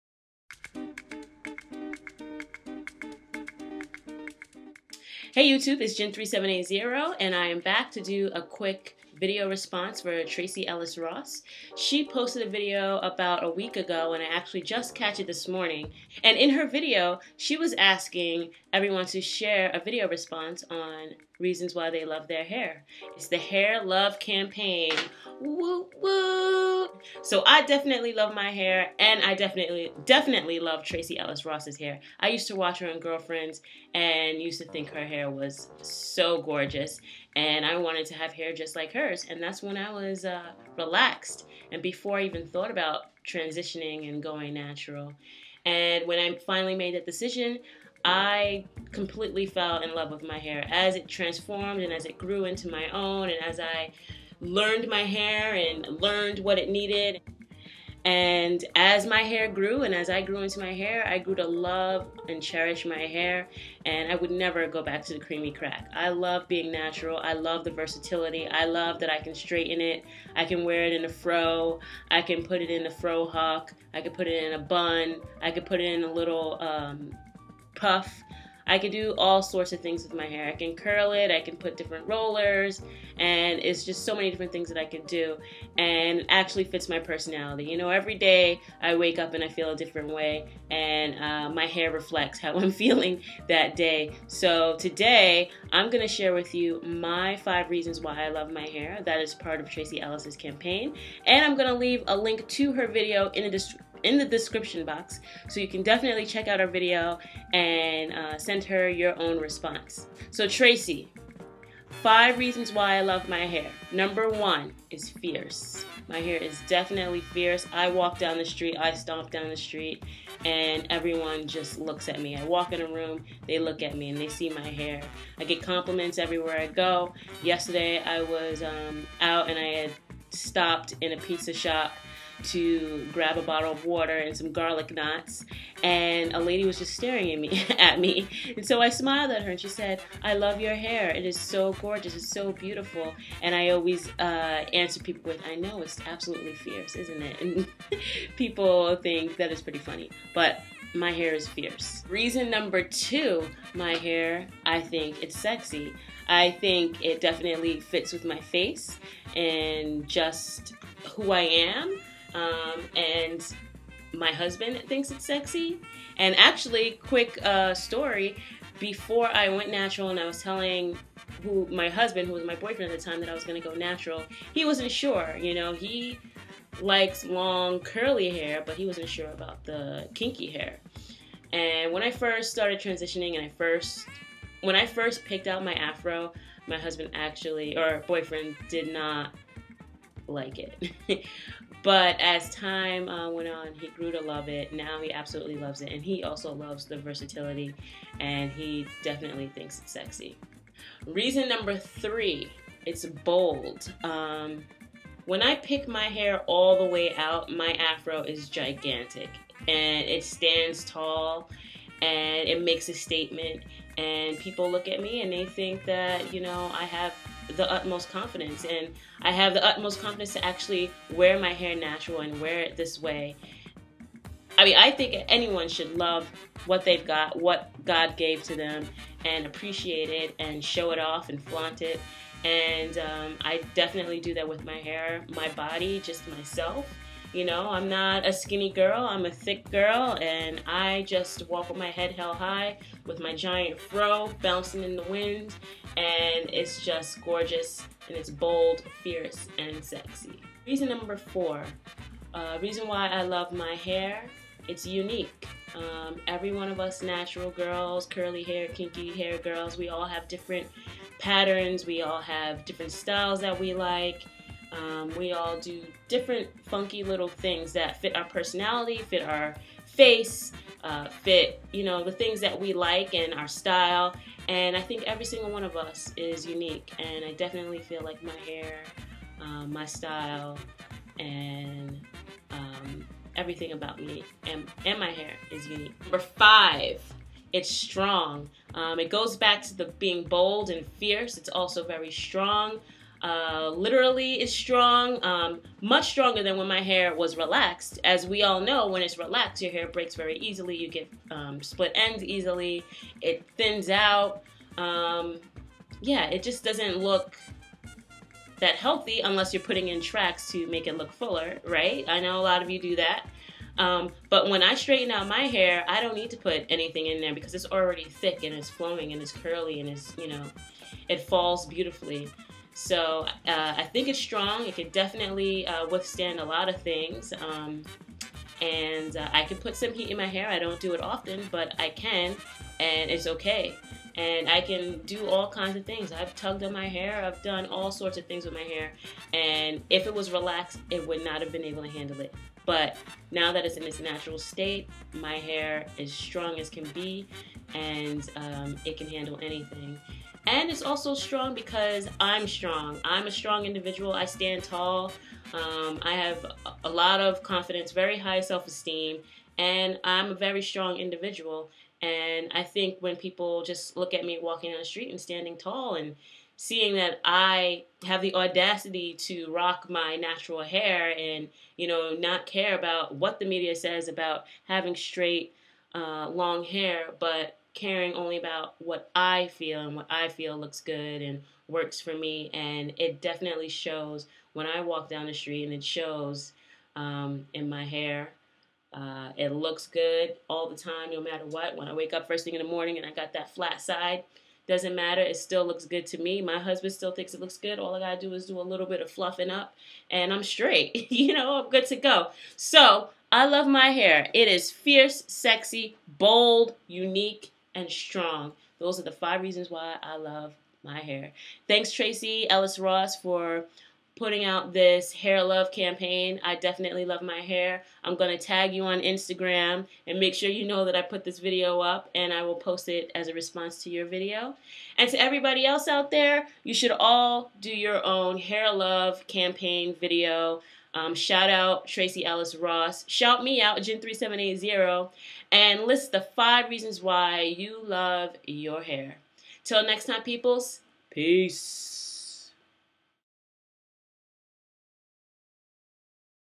Hair Love Tag: a response to Tracee Ellis Ross ;-)
Special Thanks to Tracee Ellis Ross for getting us to share our hair love!